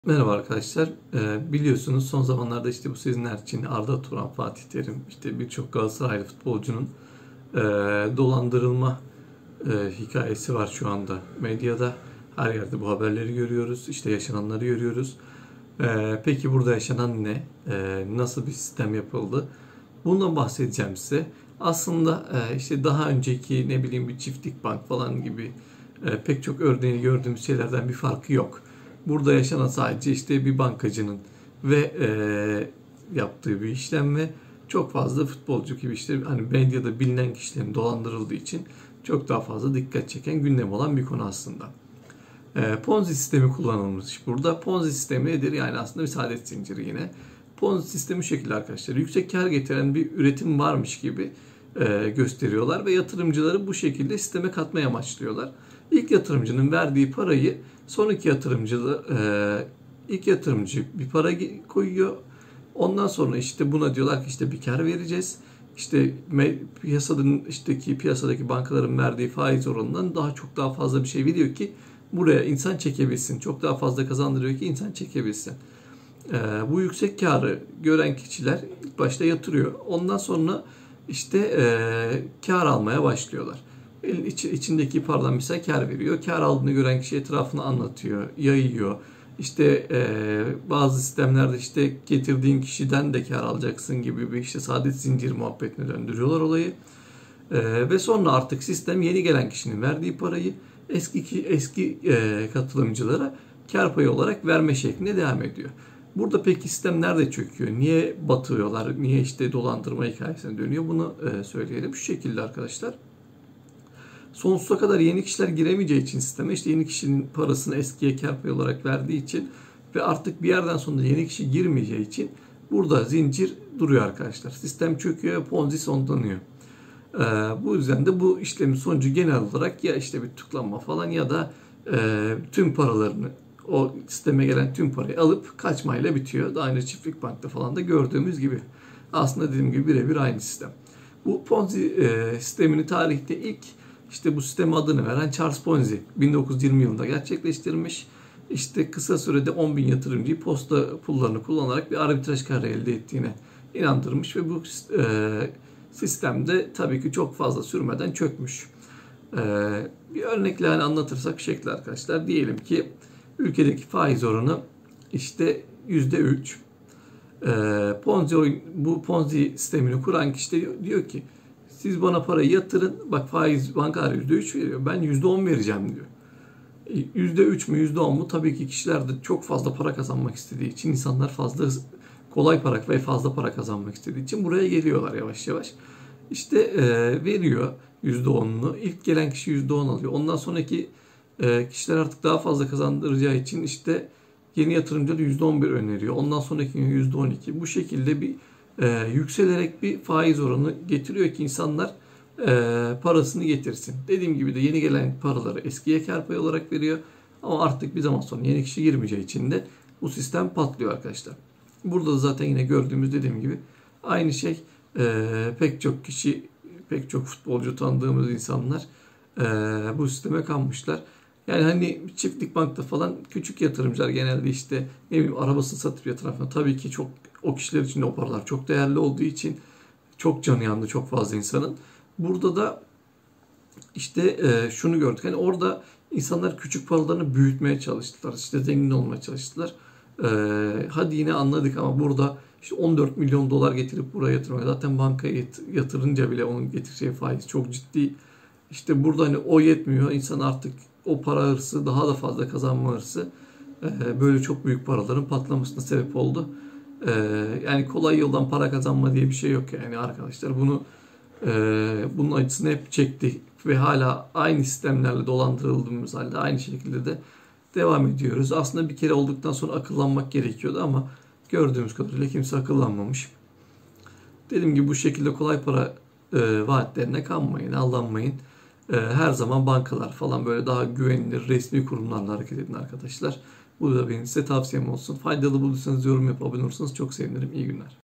Ponzi Sistemi nedir?
Seçil Erzan , arda turan emre belözoğlu gibi bir çok futbolcuyu dolandırmakla yargılanıyor. Peki nasıl bir sistem Kurdu?